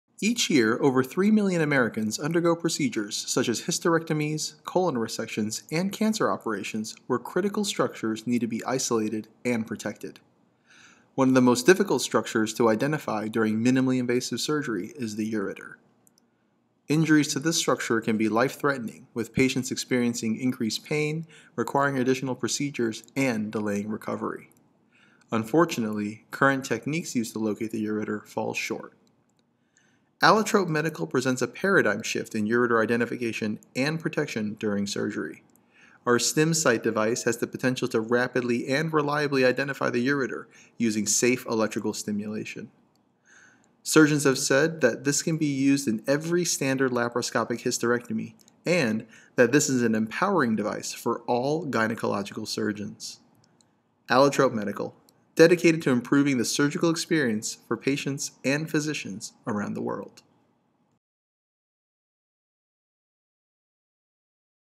Allotrope Medical
Handheld device using electrical stimulation for ureter identification during surgery